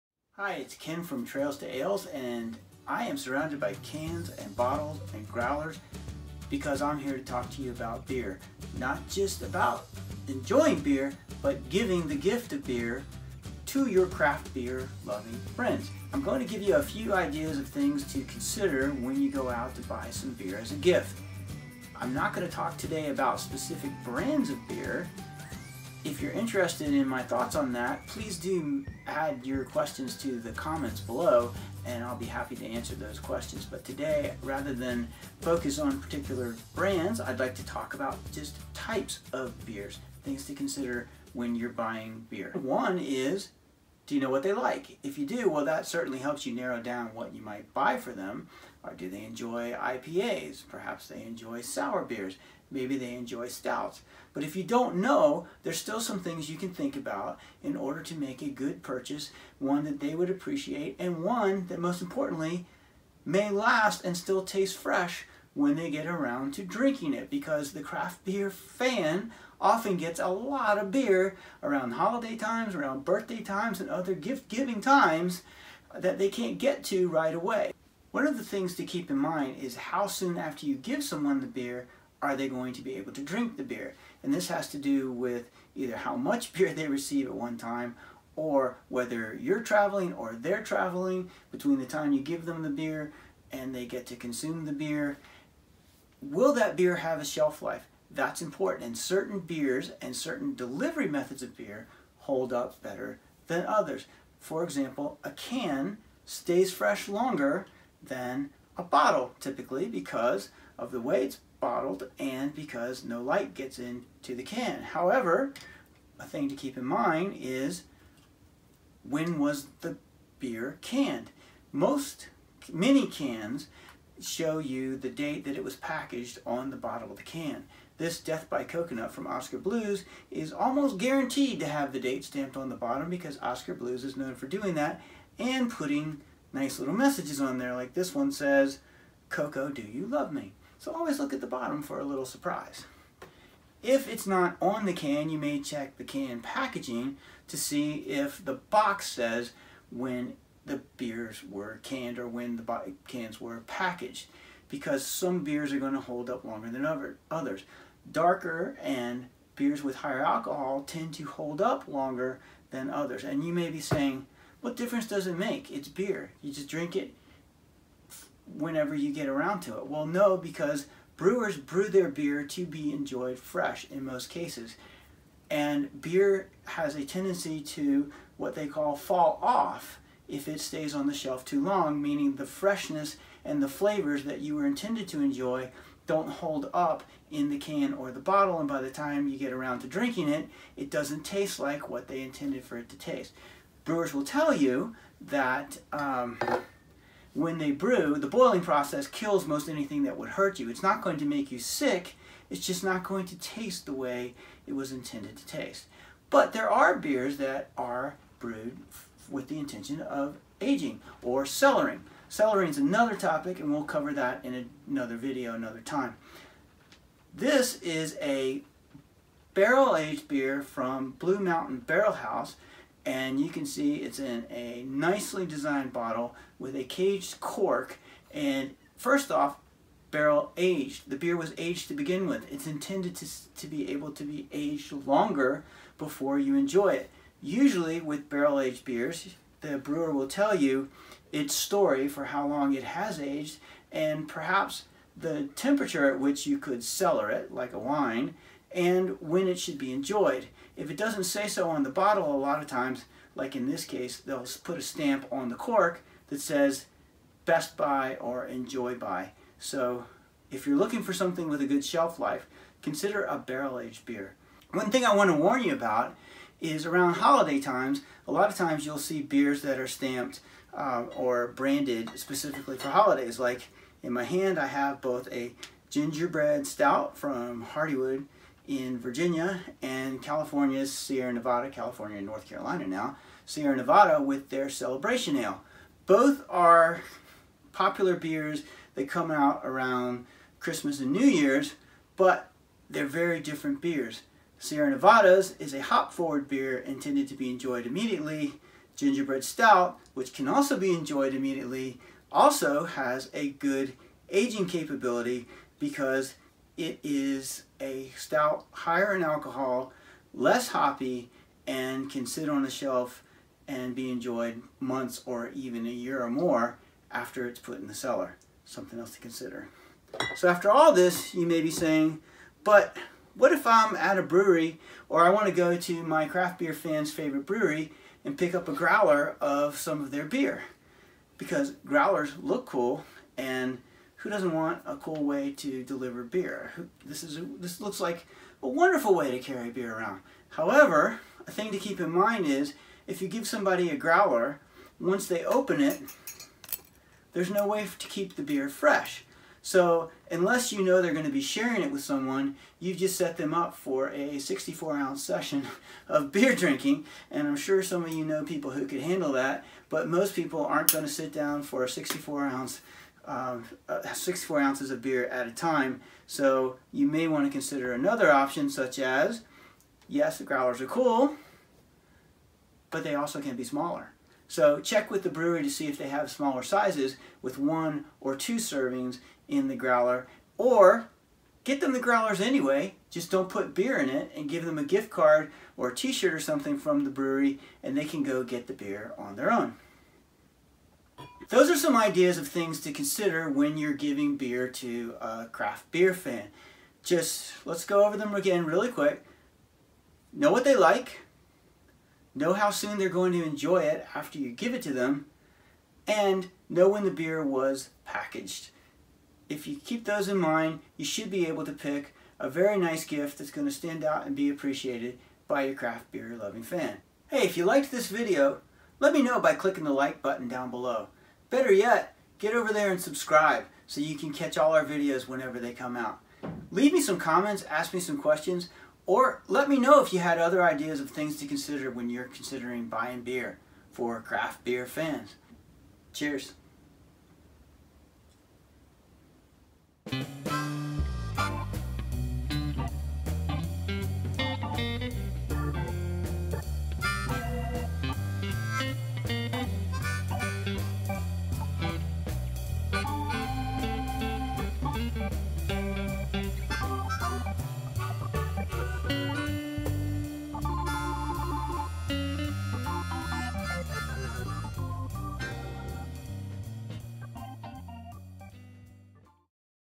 The Gift of Beer: Buying Beer for a Beer Lover
Cans or bottles? Crowler or Growler? IPA, Sour or Stout? If you're buying beer as a gift, there a few things you should keep in mind before making the purchase. This video gives you tips to make sure your choice is appreciated and enjoyed! If you ARE the beer lover, use this video to help educate those who may one day deliver a nice hoppy surprise to your door.